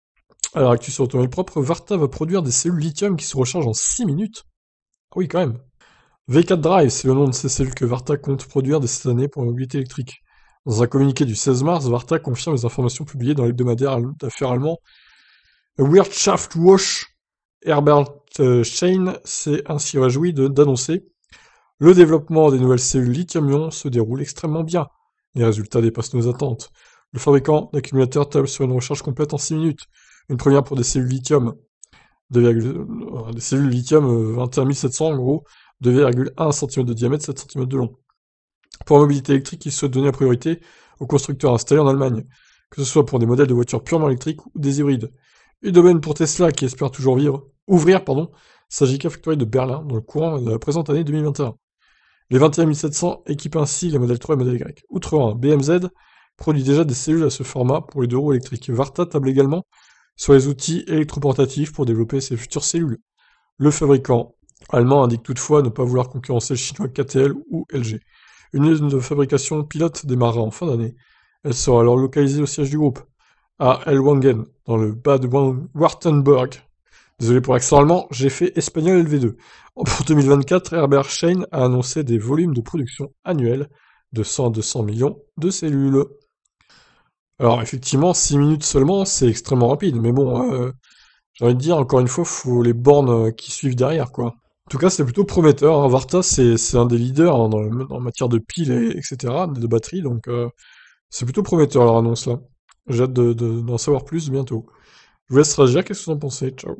Varta va lancer des cellules lithium qui se rechargent en 6 minutes seulement !
Encore faut-il les bornes qui vont avec...